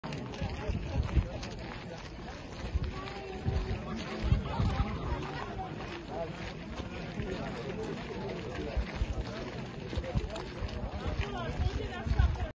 Otočme sa im chrbtom
Borisovi Kollárovi sme sa dnes na Bradle otočili chrbtom. Budeme tak robiť na každej akcii, kde budú mať dnešní vládni predstavitelia prejav.

Tak ako sa oni otáčajú národu chrbtom, budeme ich ignorovať na každom verejnom podujatí aj my.